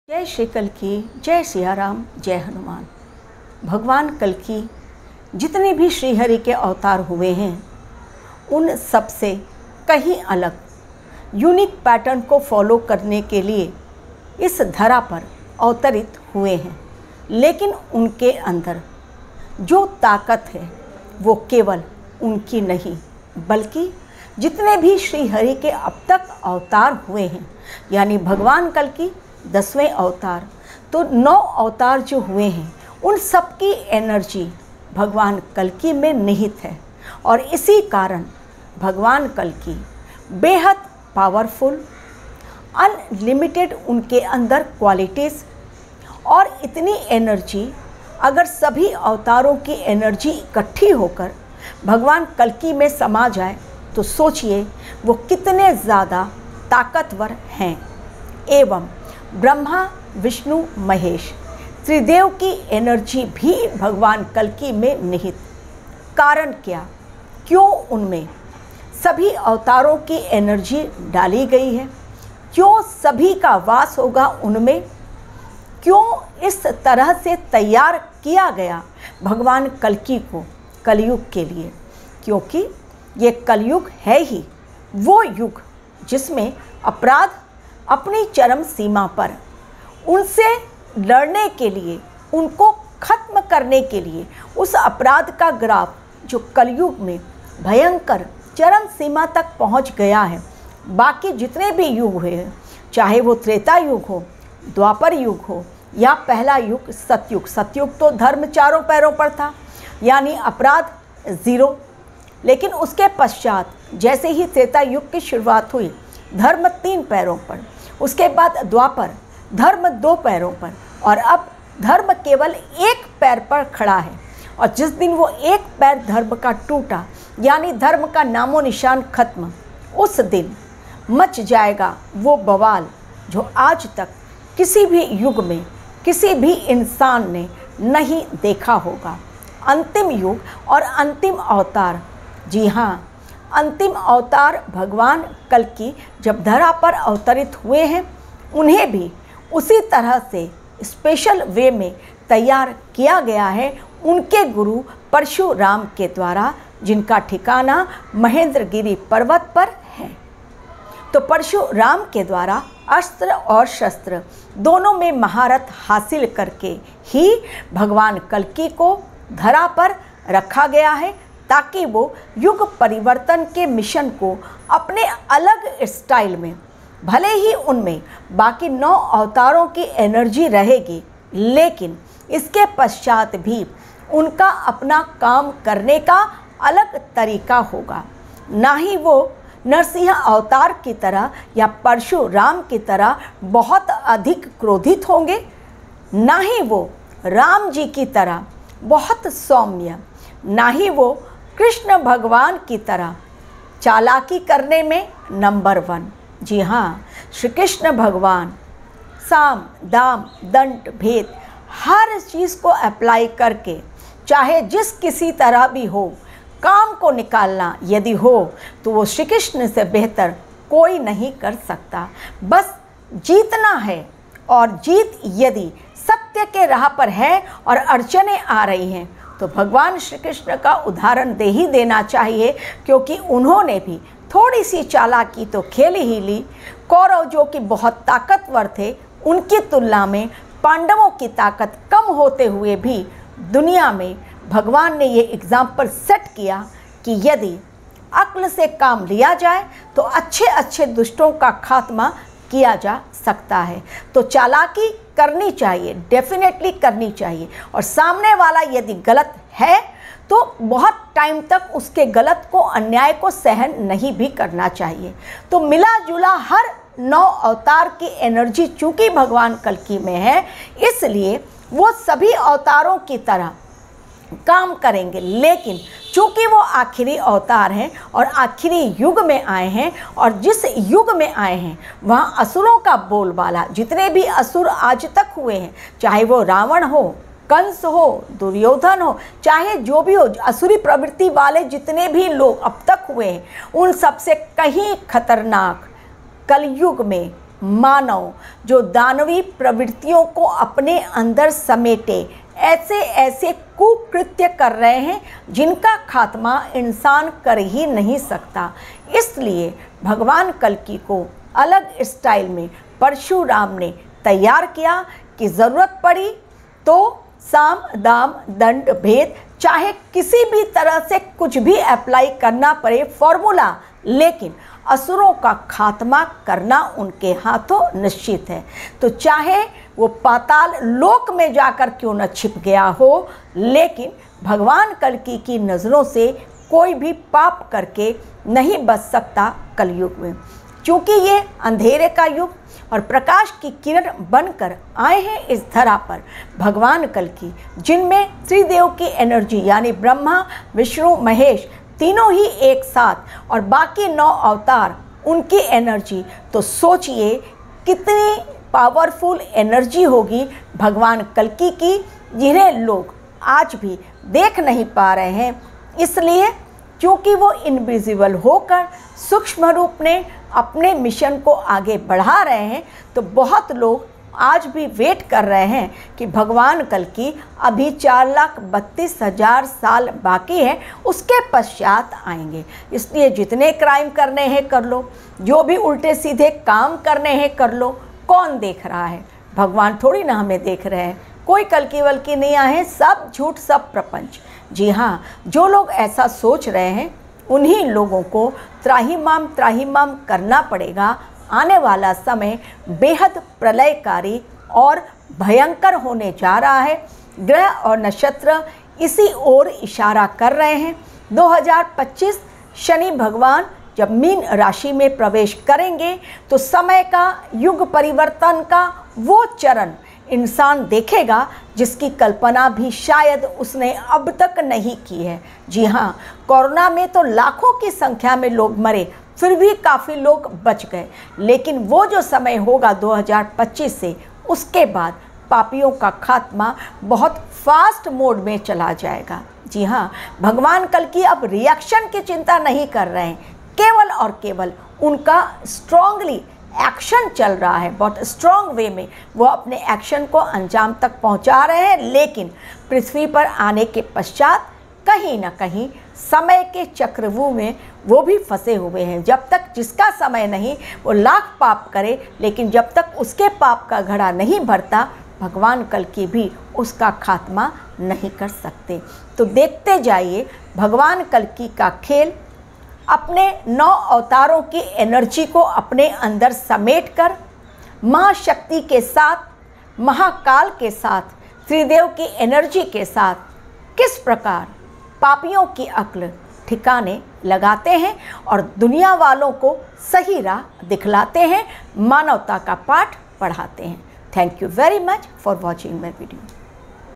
Kalki Avatar 10 in 1, why? Kalki - a mysterious Avtar
Kalki Avatar 10 in 1, why? Kalki - a mysterious  Avatar

Kalki Avatar's mysterious personality and hidden mission is very surprising. Thanks.